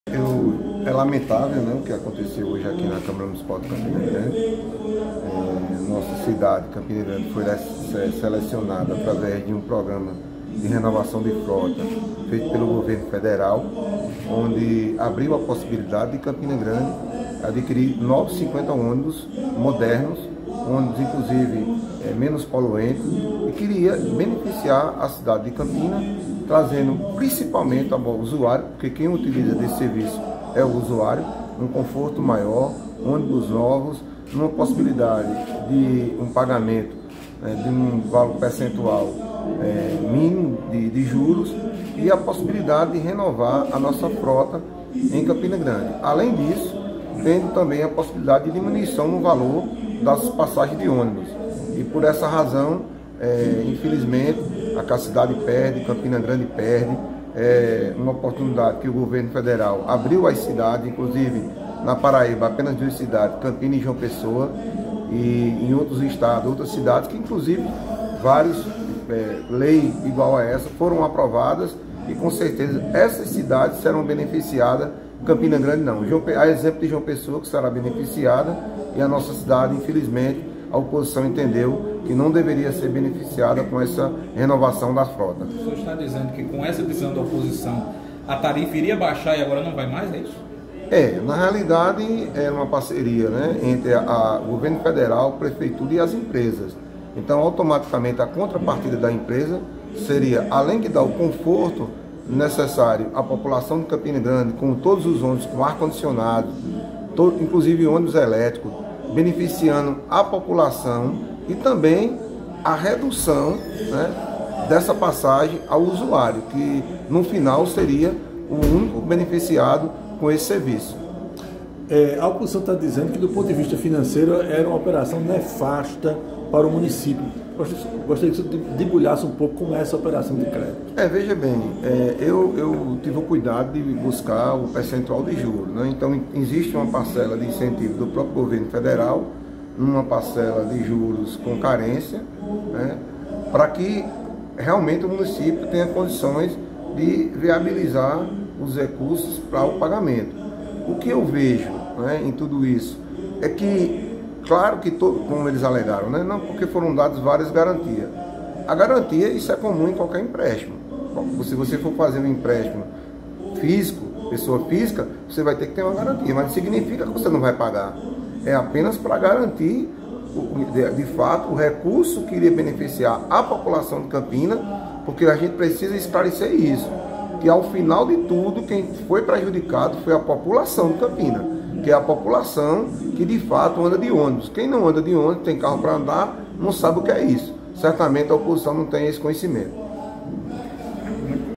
Vereador lamenta não aprovação de empréstimo da Prefeitura de Campina Grande
O vereador Luciano Breno (Avante) expressou decepção após a Câmara Municipal de Campina Grande rejeitar o pedido de empréstimo de mais de R$ 60 milhões solicitado pela prefeitura para a aquisição de novos ônibus. Segundo Luciano, a rejeição representa uma perda significativa para a cidade, que tinha a chance de receber 50 novos ônibus equipados com ar-condicionado e tecnologia elétrica.